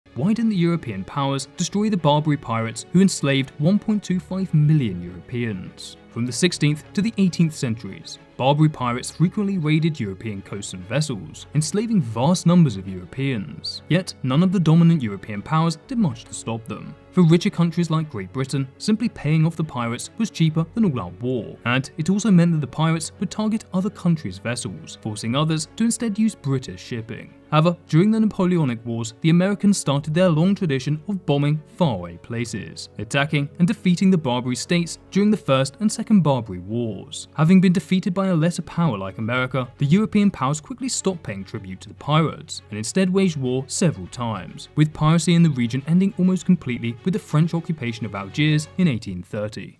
Why Didn’t The European Powers Destroy The Barbary Pirates Who Enslaved 1.25 Million Europeans?
From the 16th to the 18th century, Barbary Pirates frequently raided European coasts and vessels, enslaving vast numbers of Europeans. Yet none of the dominant European powers did much to stop them. The main reason was economic. For richer countries like Great Britain, simply paying off the pirates was cheaper than all out war, and it also meant that the pirates would target other countries vessels, forcing others to instead use British shipping. However, during the Napoleonic wars, the Americans started their long tradition of bombing far away places, attacking  and defeating the Barbary States during the First and Second Barbary Wars. Having been defeated by a lesser power like America, the European powers quickly stopped paying tribute to the pirates, and instead waged war several times, with piracy in the region ending almost completely with the French occupation of Algiers in 1830.